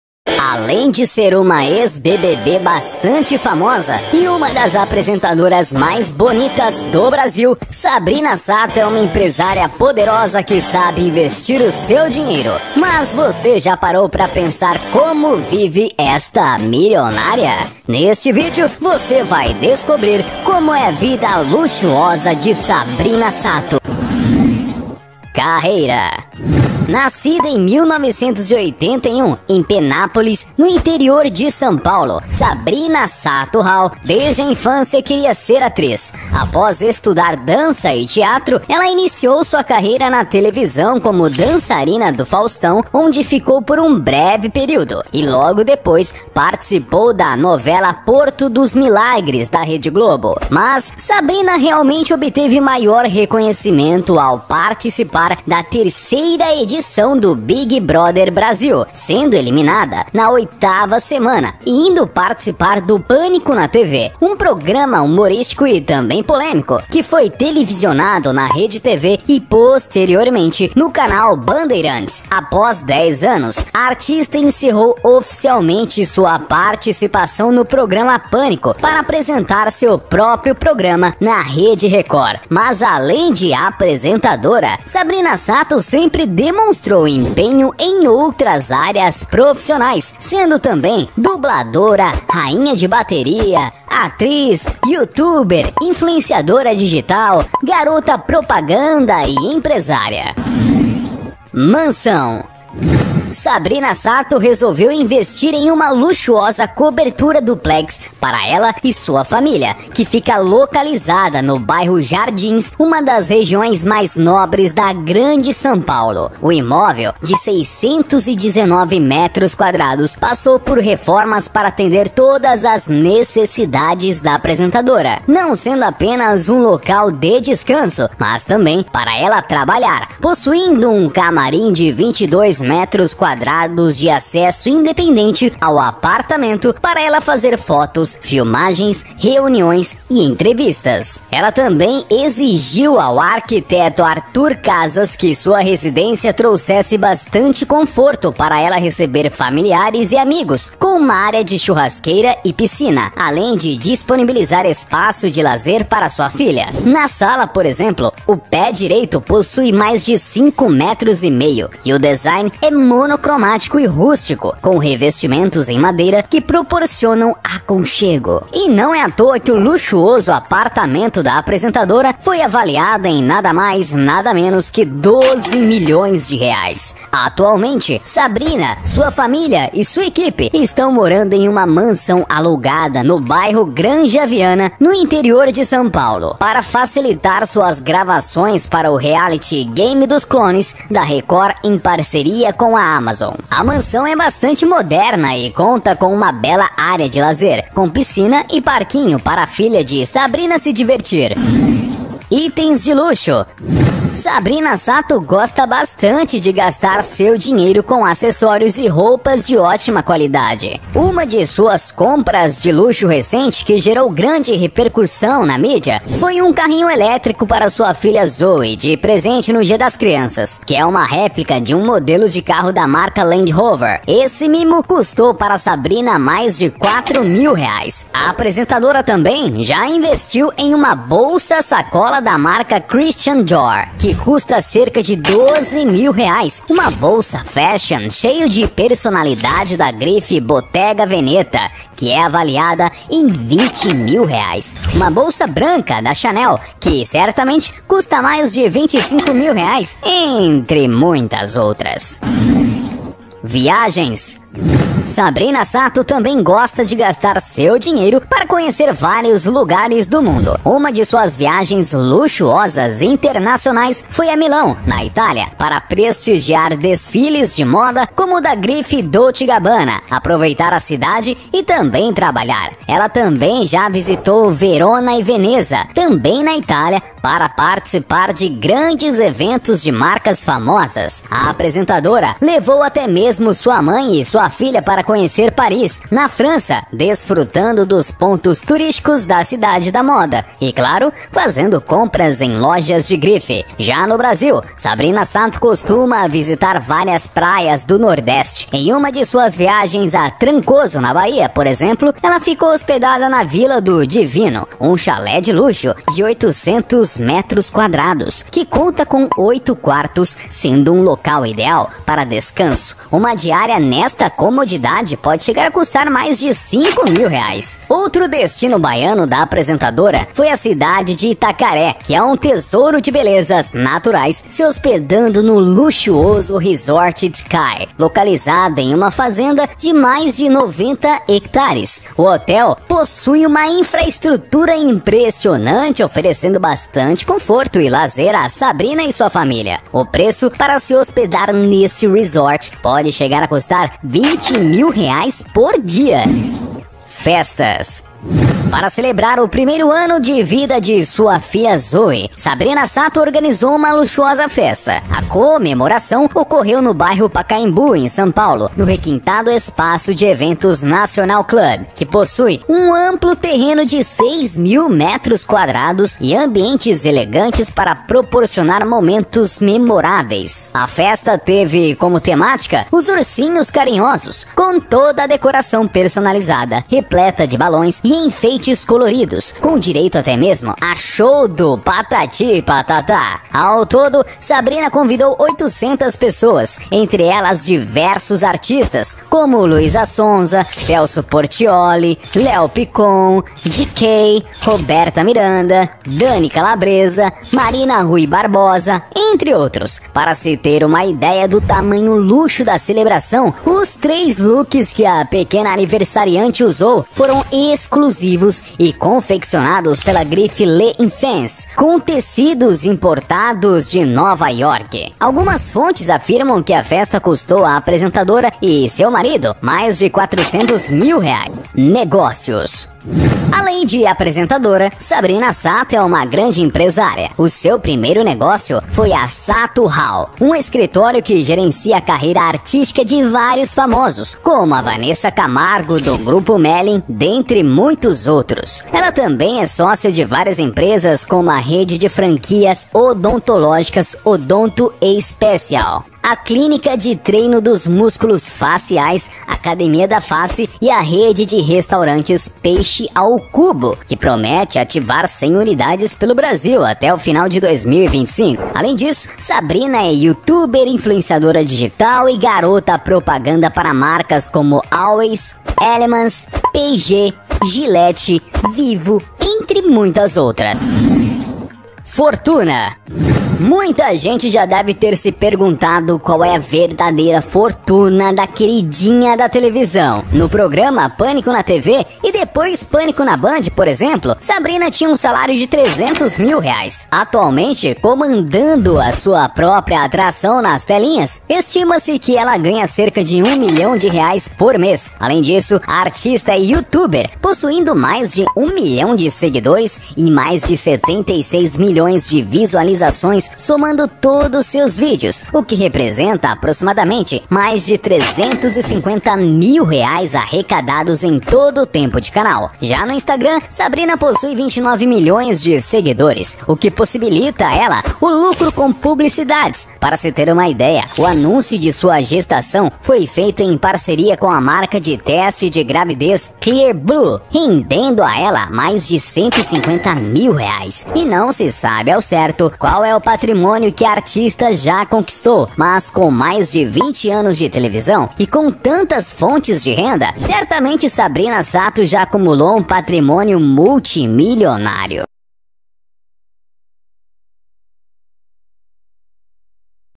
ESSA É A VIDA LUXUOSA DA MILIONARIA SABRINA SATO
Sabrina Sato Rahal é uma apresentadora e atriz brasileira. É um dos grandes destaques do carnaval no país. Ficou conhecida ao integrar por uma década o programa Pânico, tanto a versão radiofônica na Jovem Pan FM como na televisão, inicialmente na RedeTV! e depois na TV Bandeirantes. Wikipédia
Nascimento: 4 de fevereiro de 1981 (idade 40 anos), Penápolis, São Paulo
Altura: 1,7 m
Parceiro: Duda Nagle (desde 2016)
Filhas: Zoe Sato Nagle
Irmãos: Karina Sato, Karin Sato